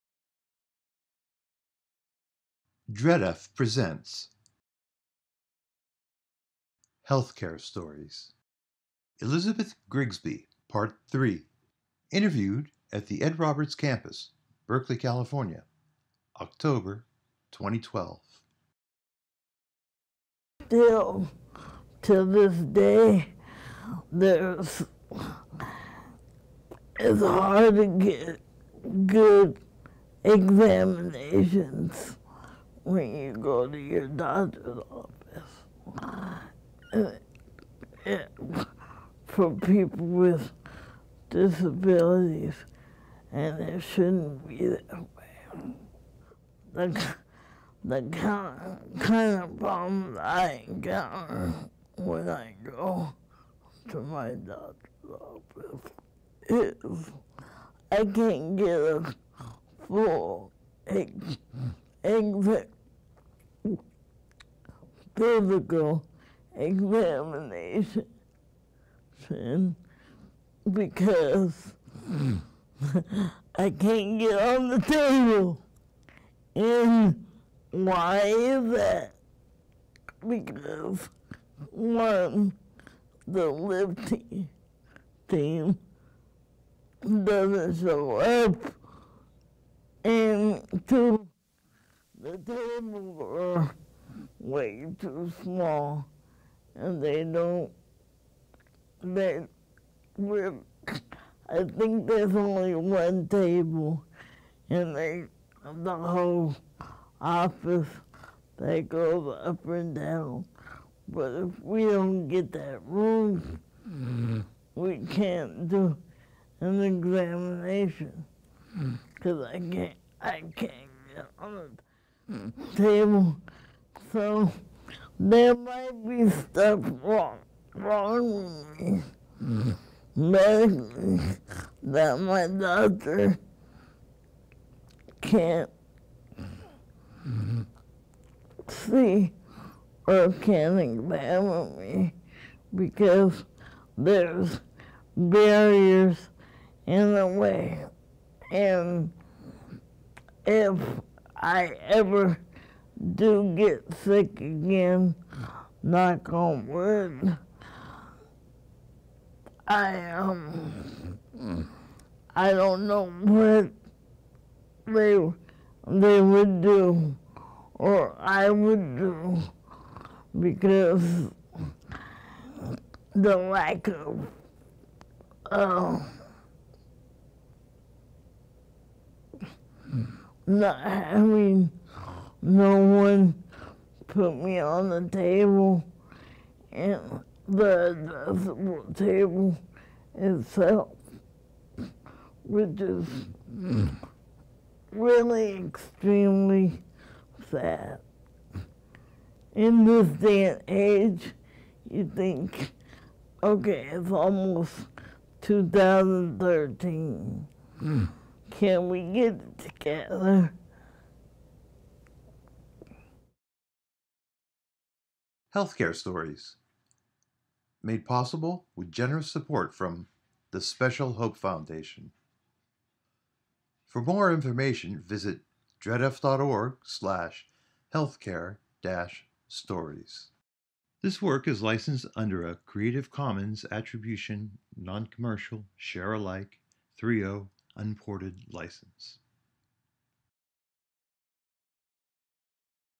HEALTHCARE STORIES - Elizabeth Grigsby Part 3
Elizabeth Grigsby lives in San Francisco, California. An advocate with a non-profit organization, she has cerebral palsy and uses a motorized wheelchair.